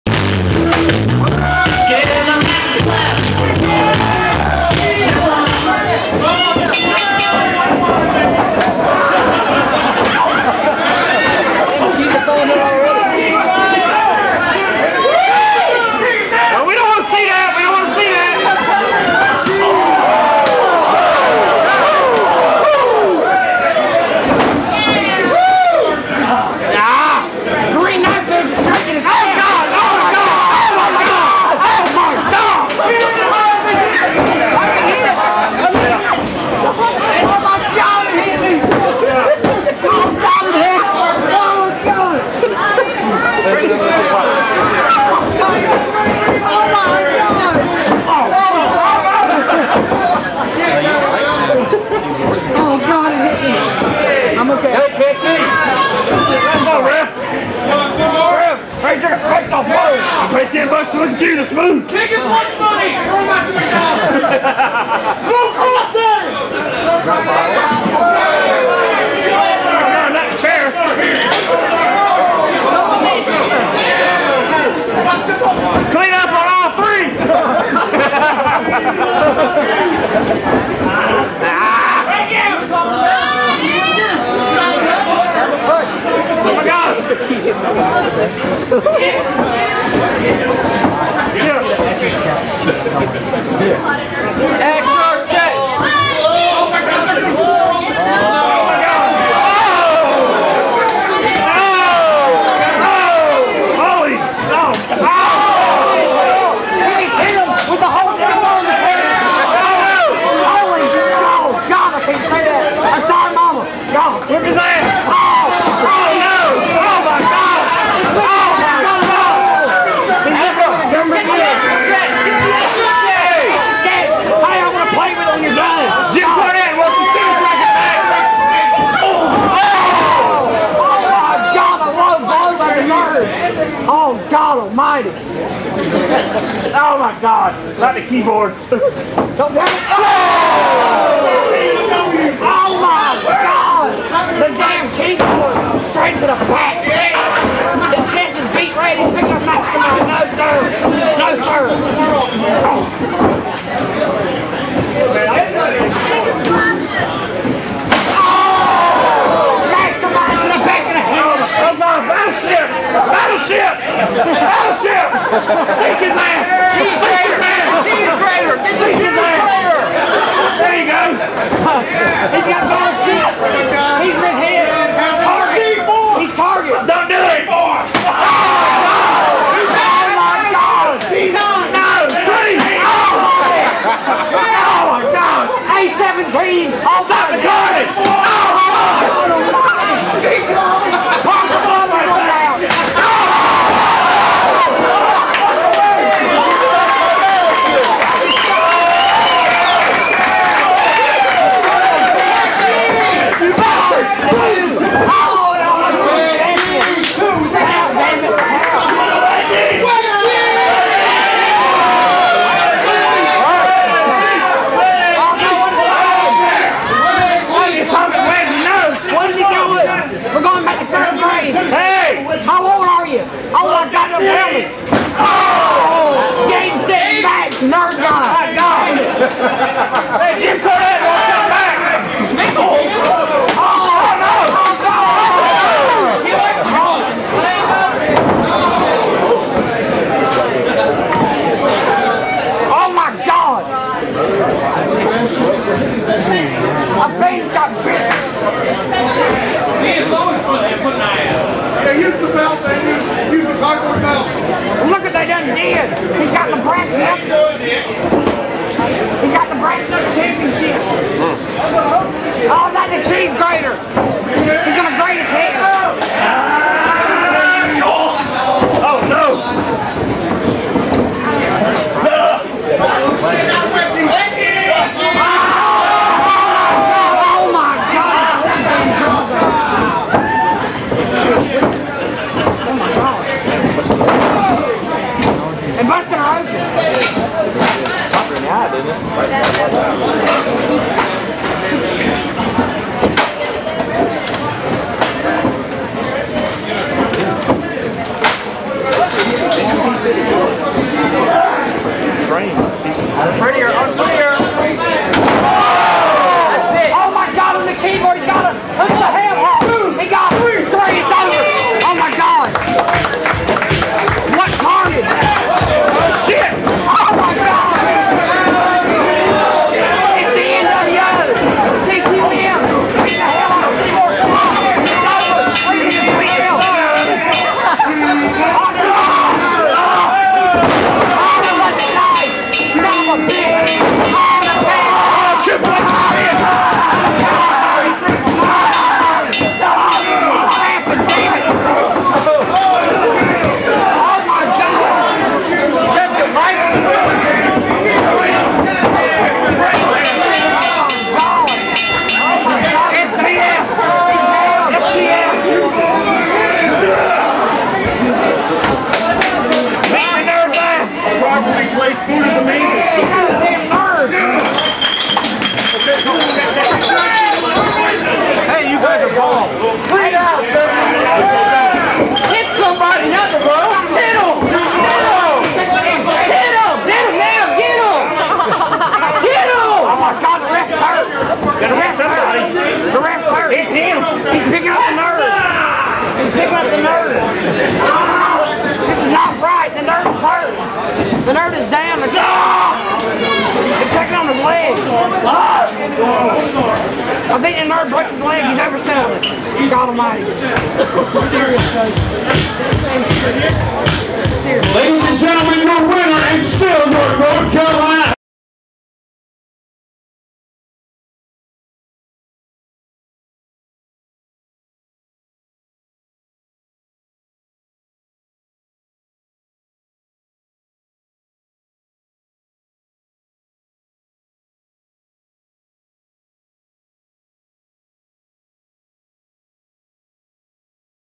SCW 5/30/2002: Silicon Valley Death Match: Seymour Snott vs. Mervin Snead
From Southern Championship Wrestling May 30, 2002 in Raleigh, North Carolina.